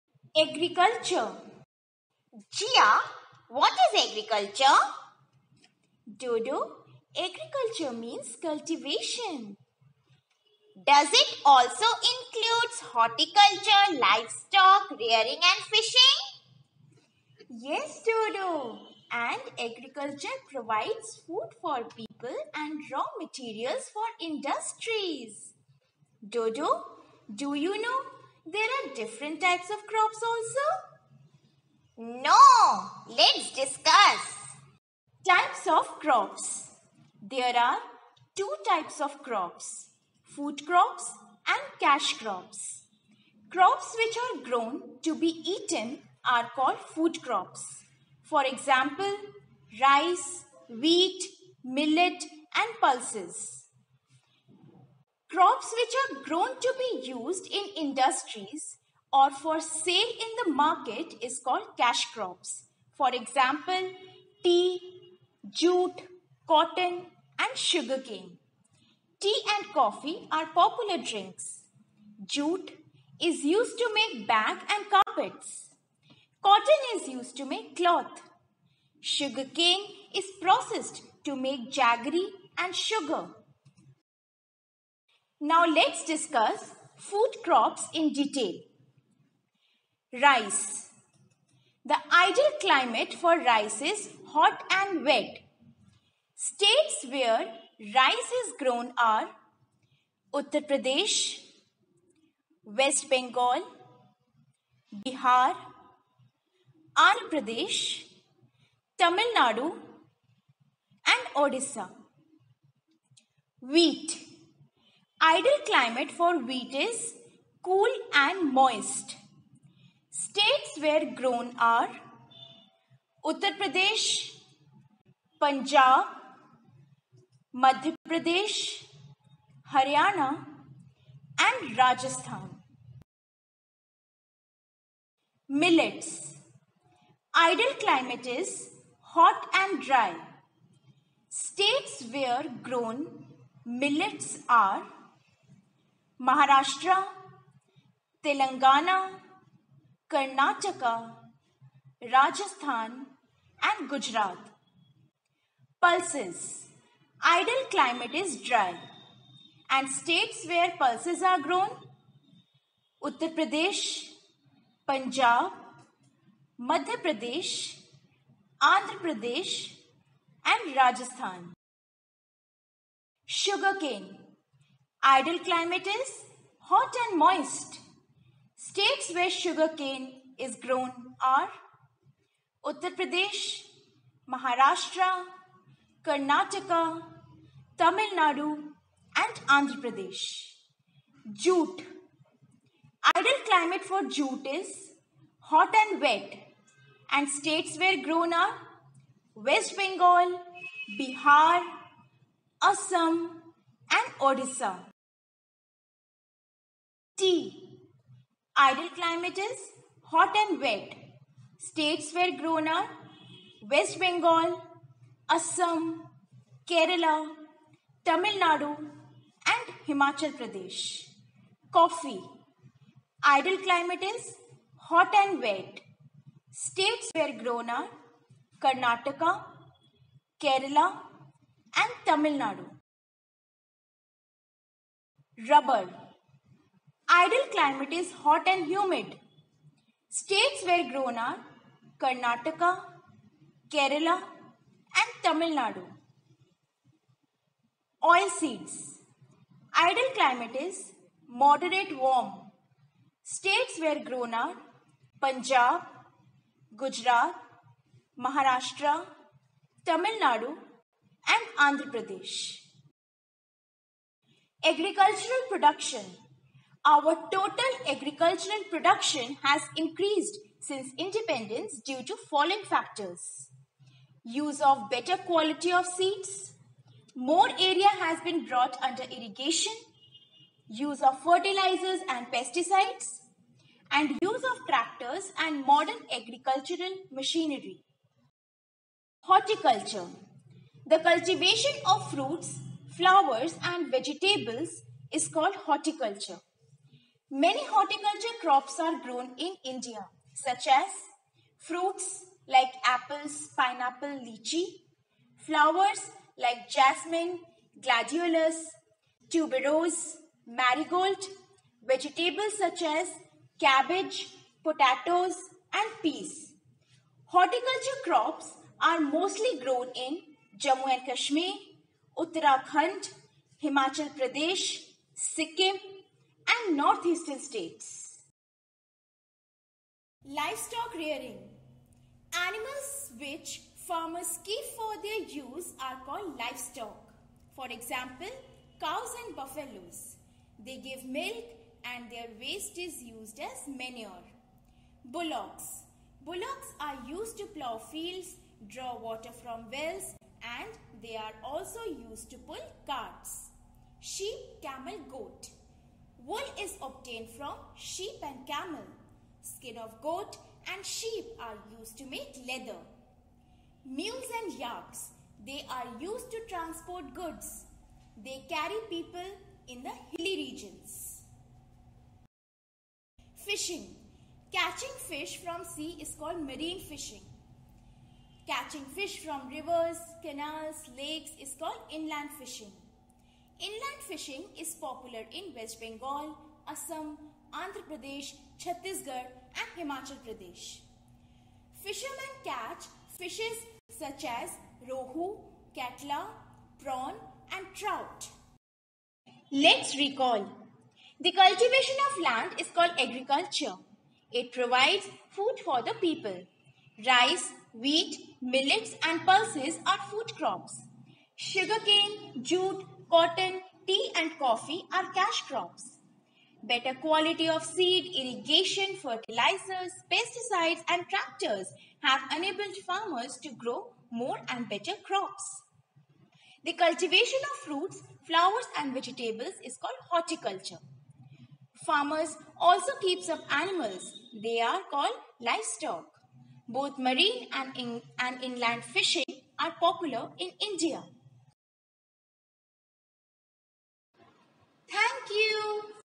Agriculture l Types of crops l food crop Cash crops l Horticulture, Animal rearing, Fishing
Do you know what is Agriculture, Do you know about different types of crops? Yes! there are two types of crops FoodCrops and CashCrops. We will learn about the ideal climate needed for these crops and in which states these are grown. Apart from AgriculturalProduction, there are other things like Horticulture, AnimalRearing, and Fishing. Let's learn about all these and much more.
Aptitude Content